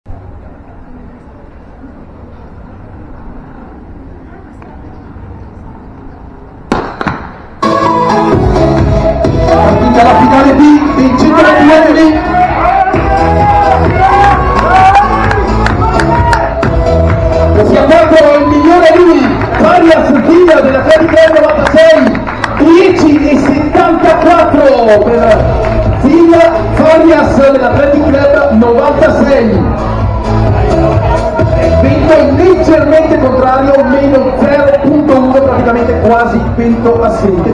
2° PERGINE MEETING 2024 - m 100 men (Final 2)
Final 2 - 100m MEN
C.S. Pergine Valsugana - 6 Jul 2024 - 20:06 - Wind: -0.1
1  ZIN Farias ATHLETIC CLUB 96 ALPERIA 10.73
2  PAGANELLI Filippo ATLETICA 85 FAENZA 10.84
3  CRISCIONE Andrea ATLETICA CLARINA TRENTINO 11.16
4  ENDRIZZI Marco ATL. VALLI DI NON E SOLE 11.23 SB
5  LASKO Stiven ATLETICA CASCINA 11.27
Galati Alessandro A.S.D. ATLETICA FUTURA ROMA DNS